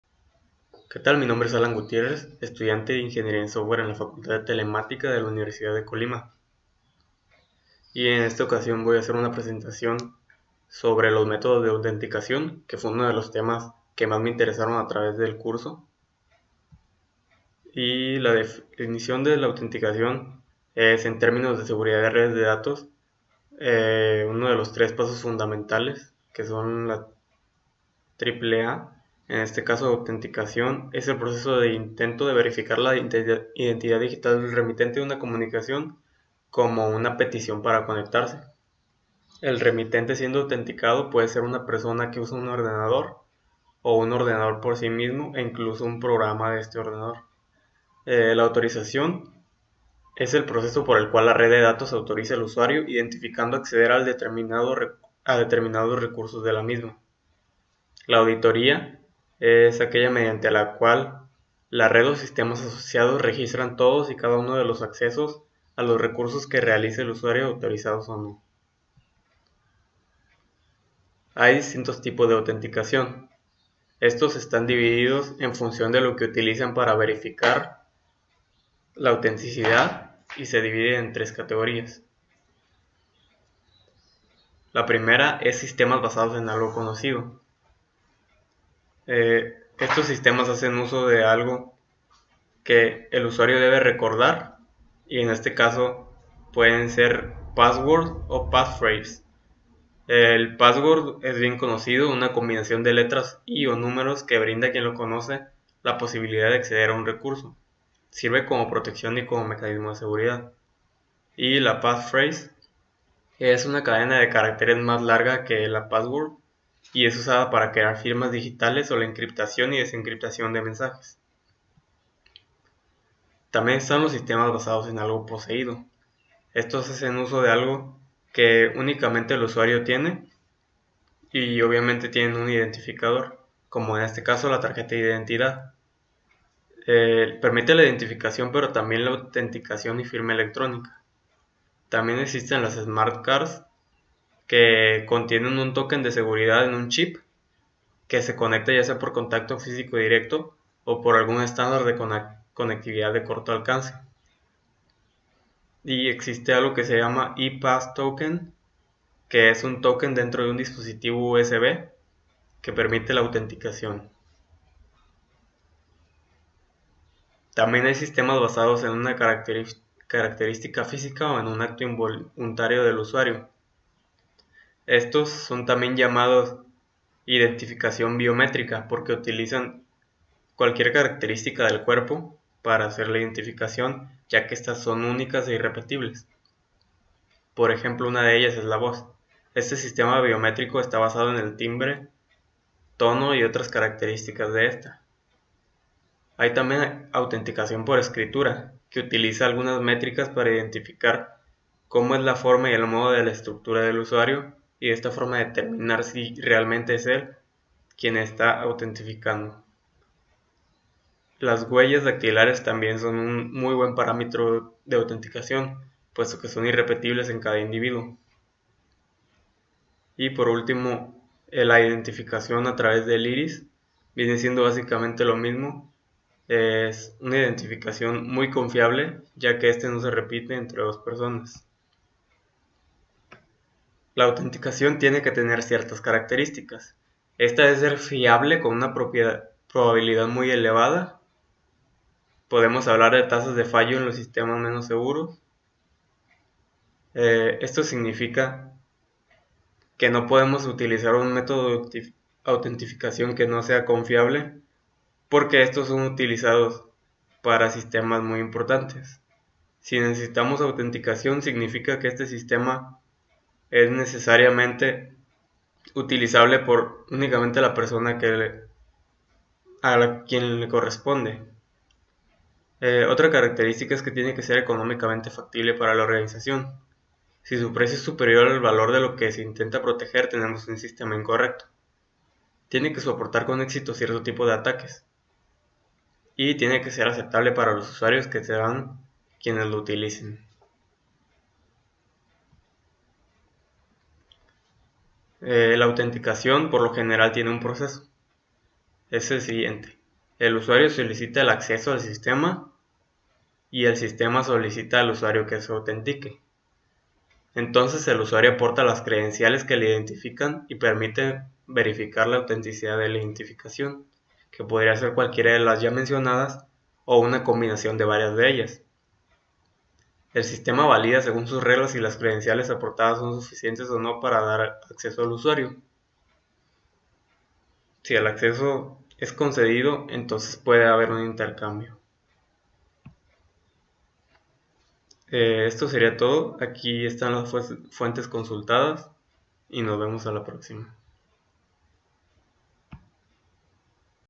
Autenticación
Descripción de algunos de los tipos de autenticación que existen en los sitemas informaticos y en que consisten.